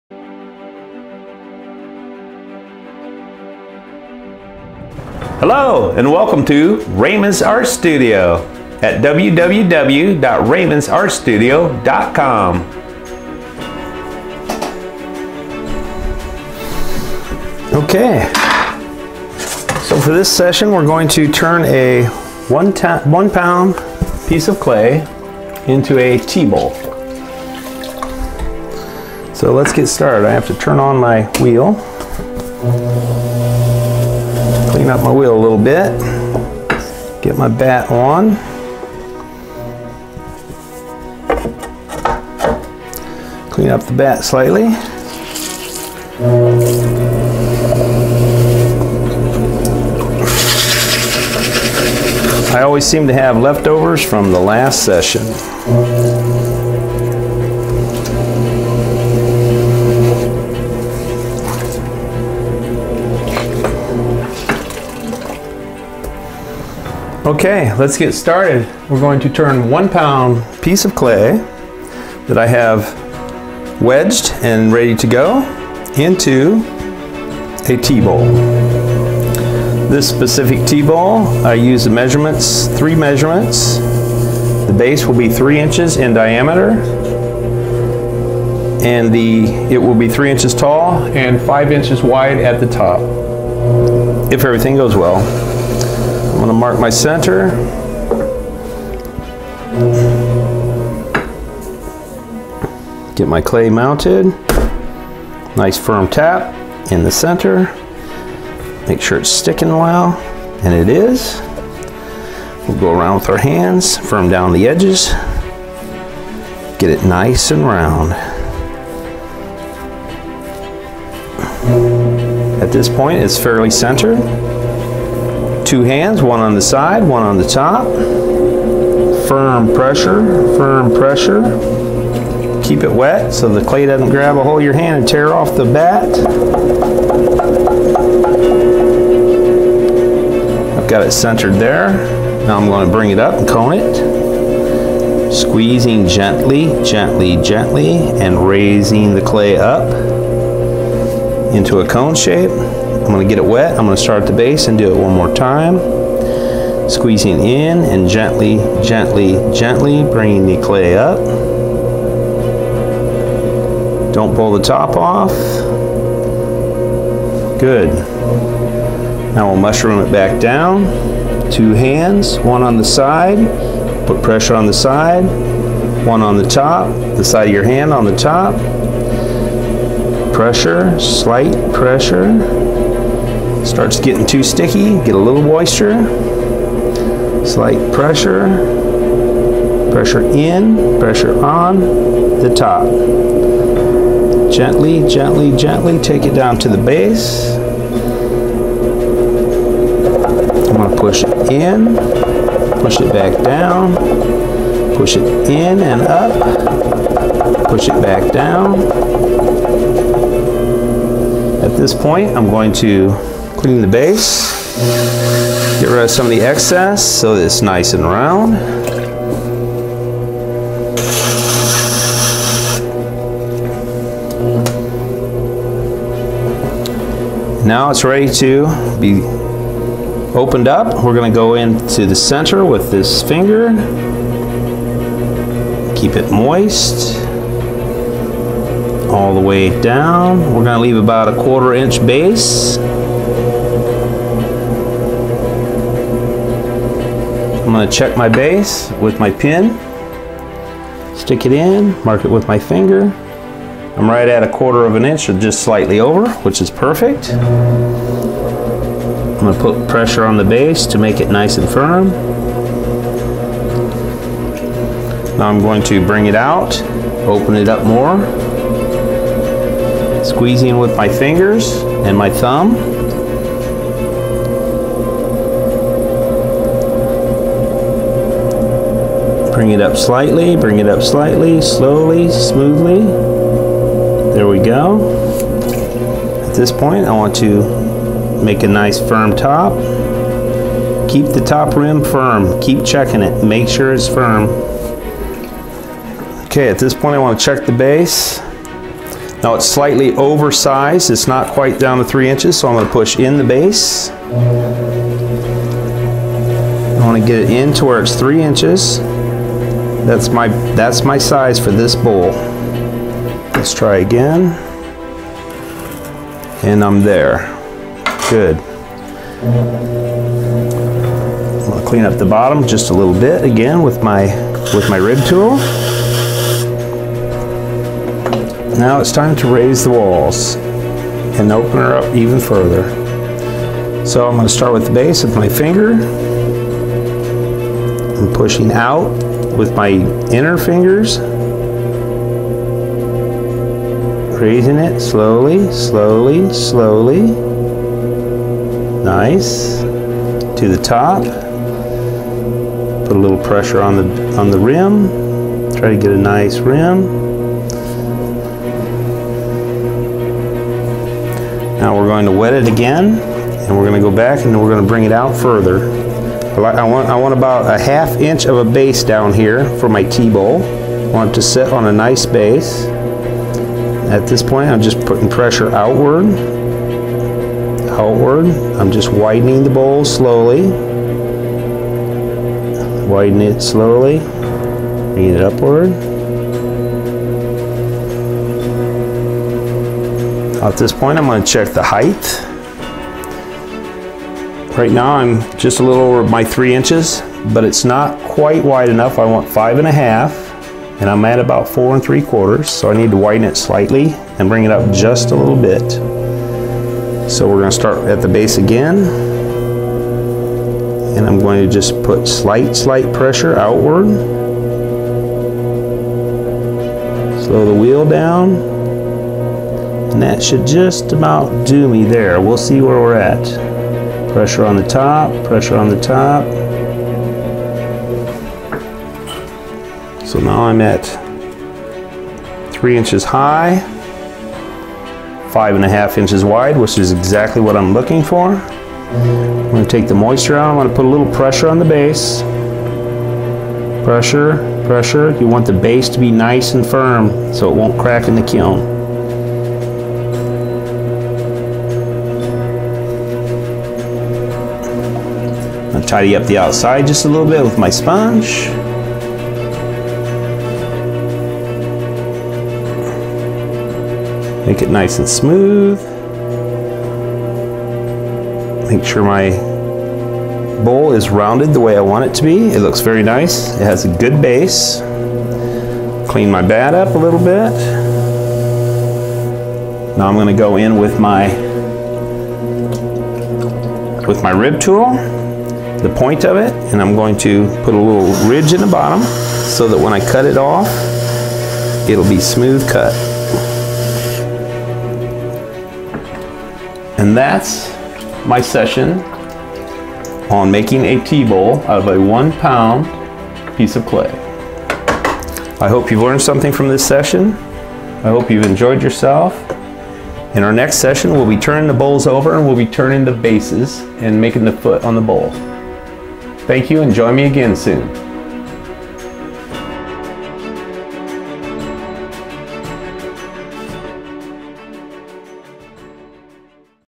Turning a one pound Tea Bowl tutorial
Raymond shows how to turn one pound of clay into A small tea bowl.  This video was made to teach people new to turning on the wheel, the basics of turning a small bowl.